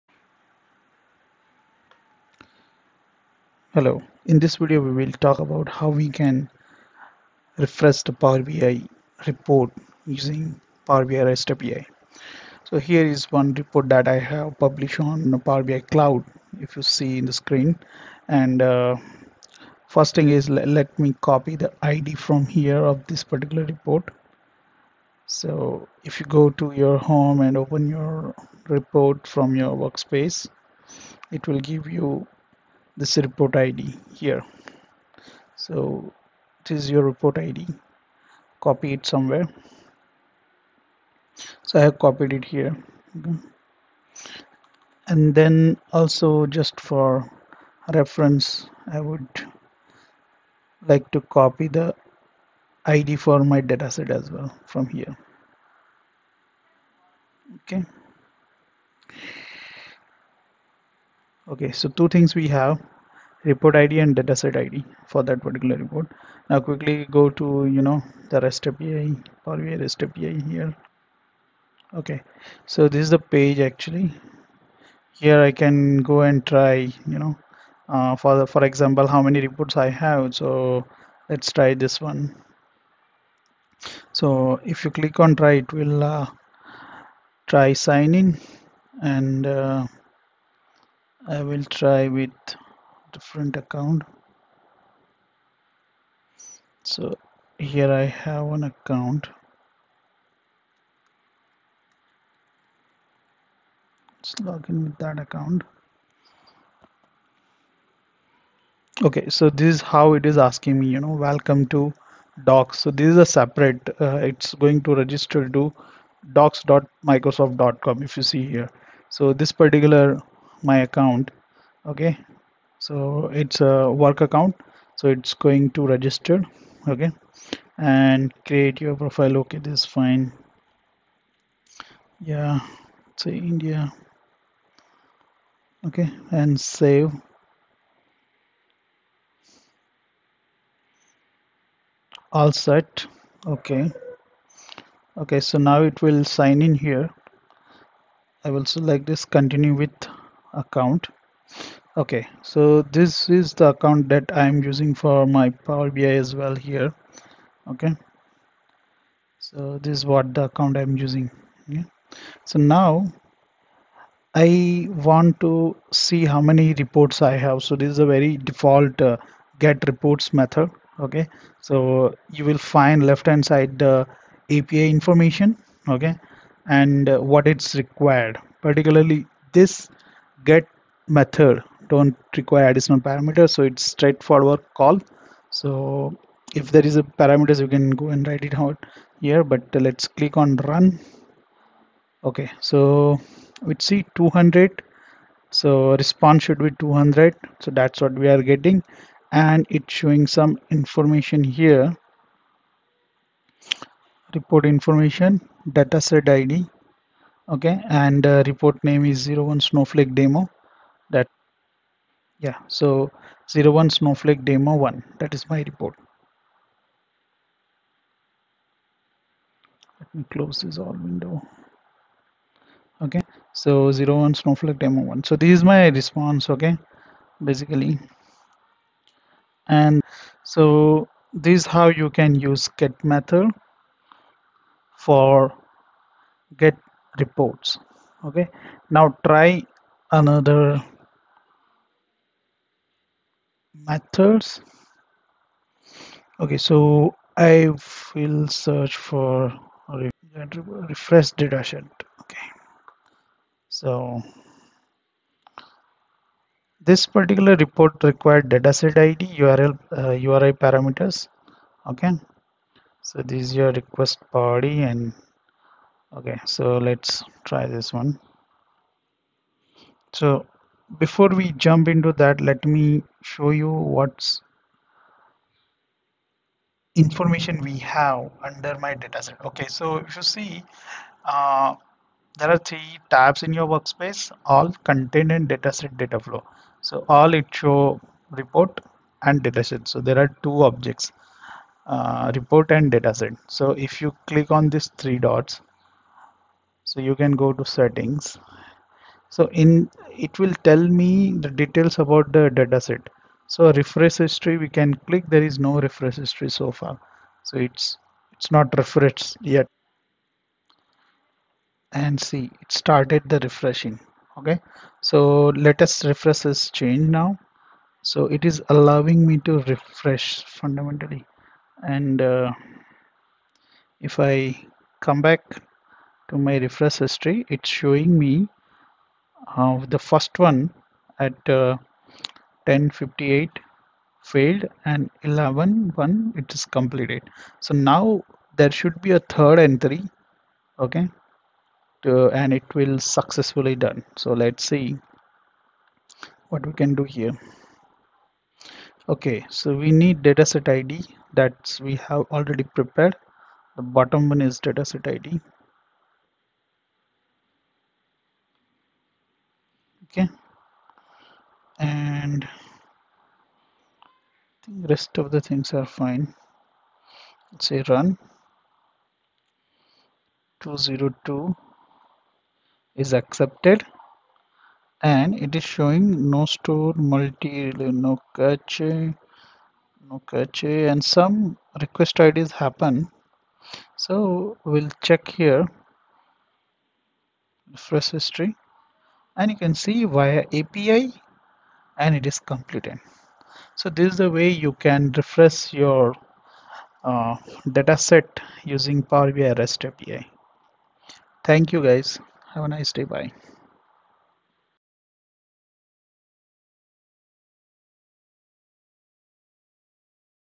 HOW TO REFRESH POWER BI REPORT USING POWER BI REST API ?
THIS VIDEO WILL EXPLAIN HOW WE CAN REFRESH THE PUBLISHED POWER BI REPORT BY CALLING POWER BI REST API...!!!